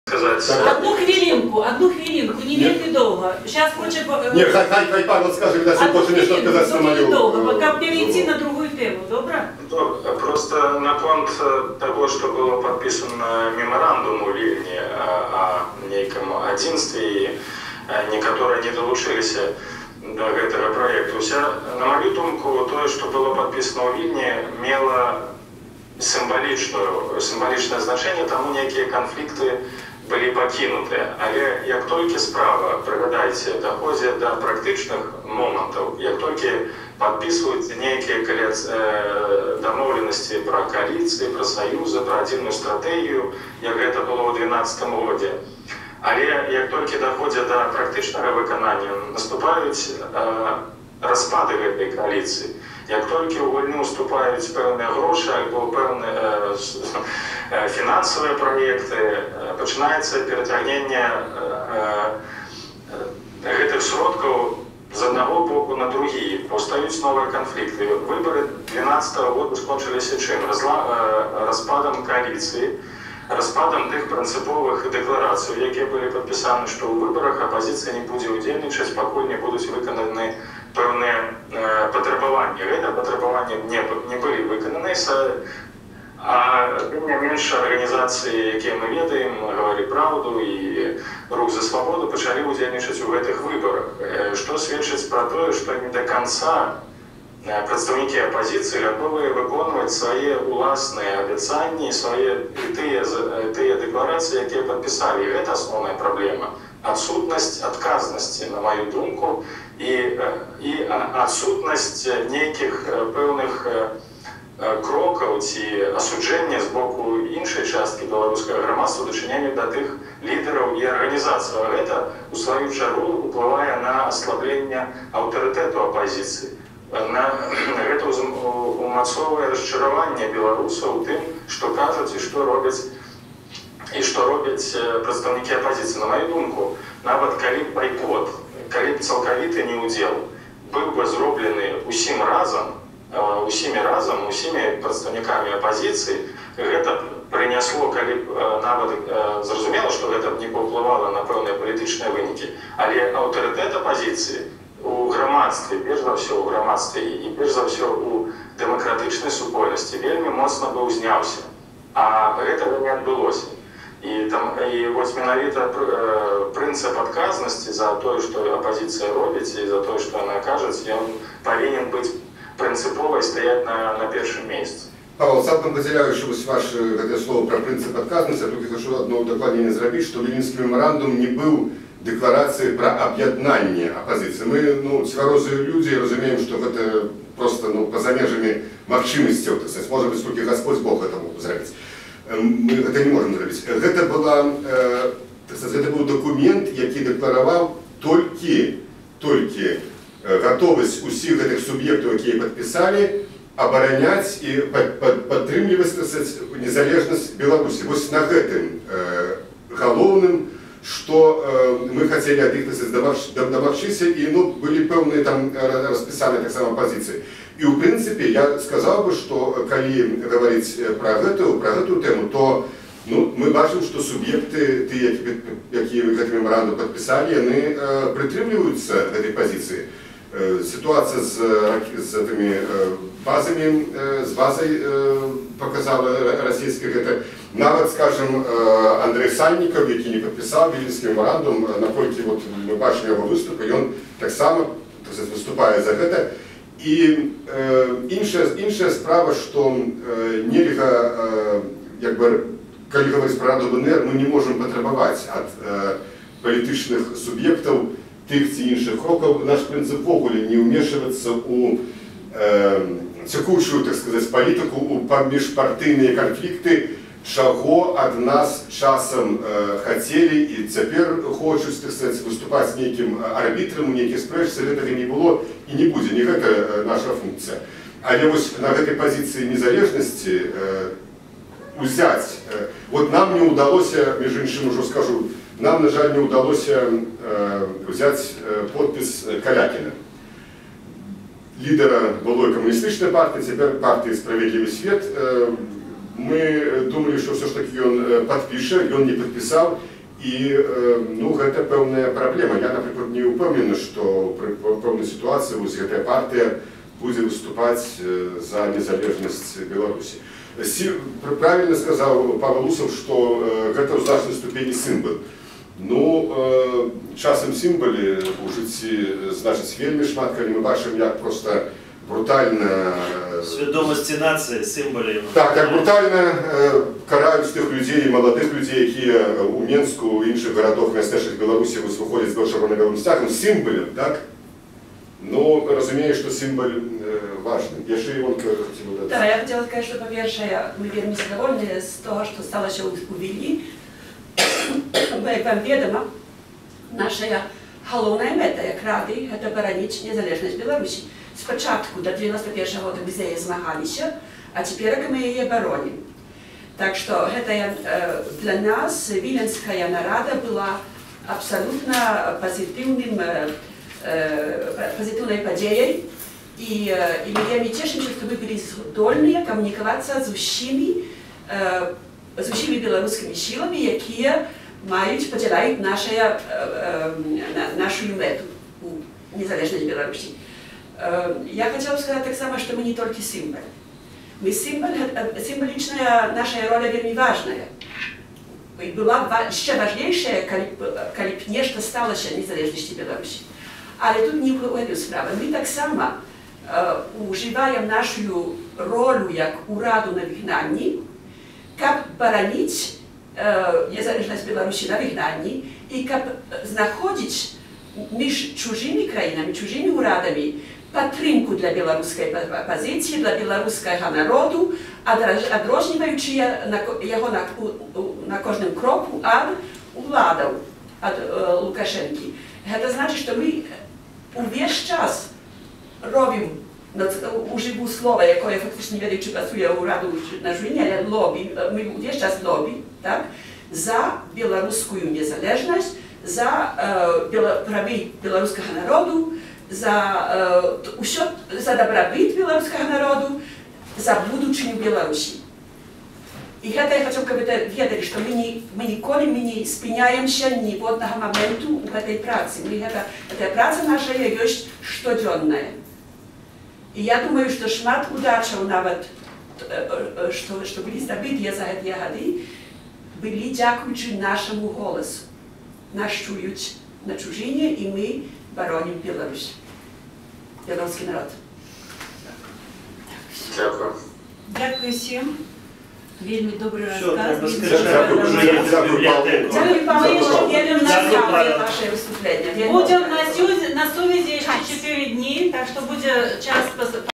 Аб дзейнасці Рады БНР
Запіс рабочай сустрэчы Рады БНР з прадстаўнікамі беларускай дыяспары ў Каліфорніі. 2013 год.

Частка 6

Сувязь з беларускай супольнасцю па Skype